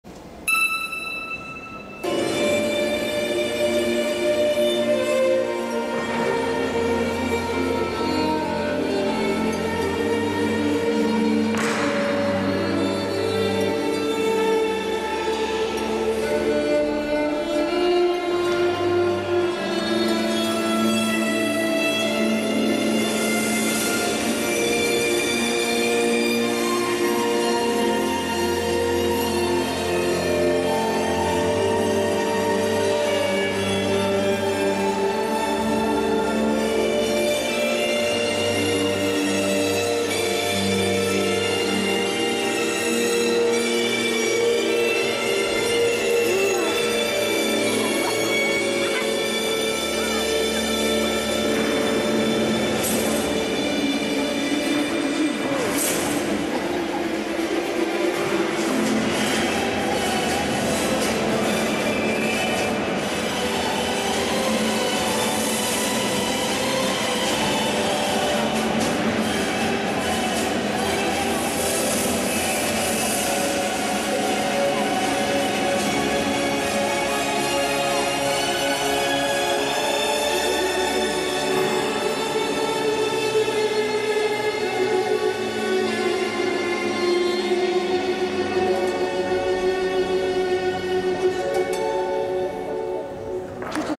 Васильевых Лиза лента
Чемпионат города Нижнего Новгорода по художественной гимнастике 27-28 сентября 2012 г. Соревнования проходят в здании Училища Олимпийского резерва